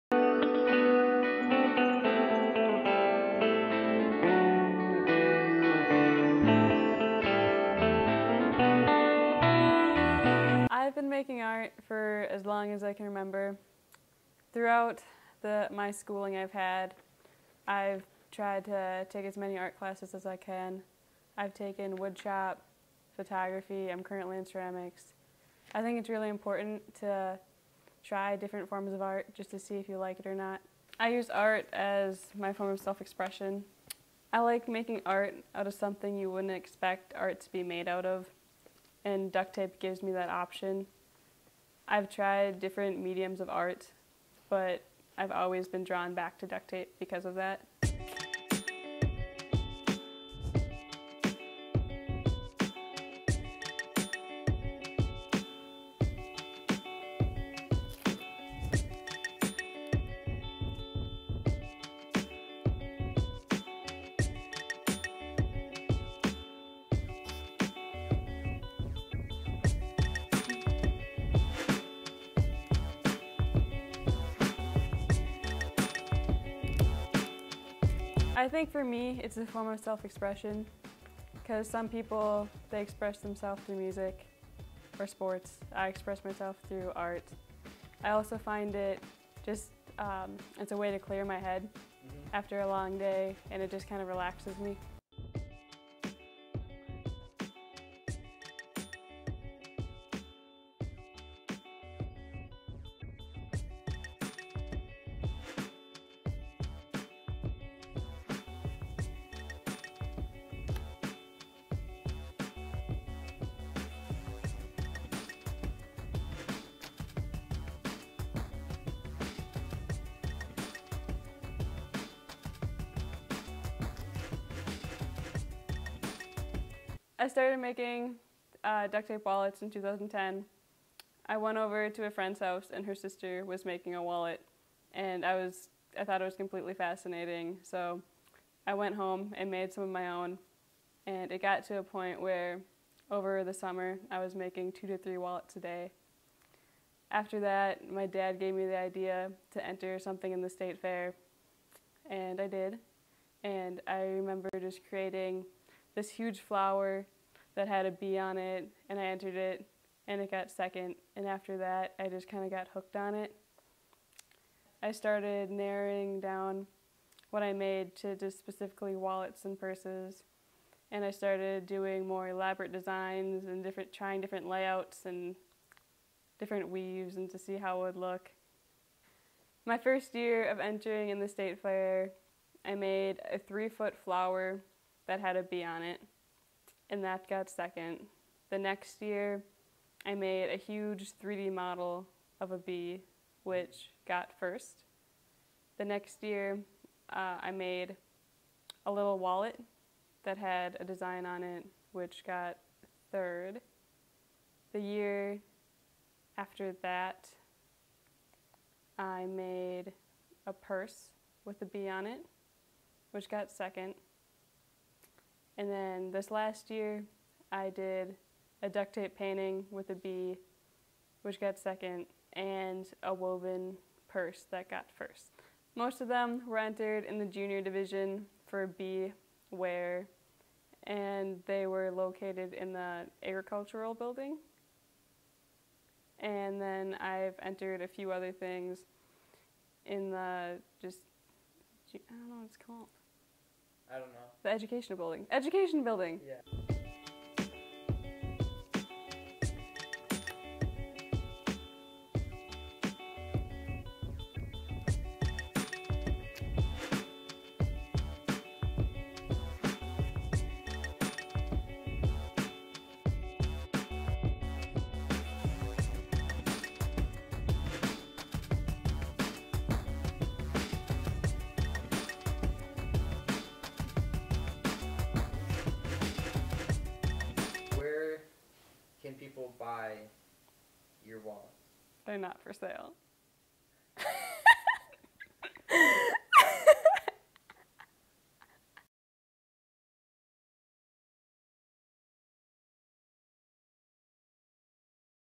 Duct Tape Artwork
The history of Rebecca's duct tape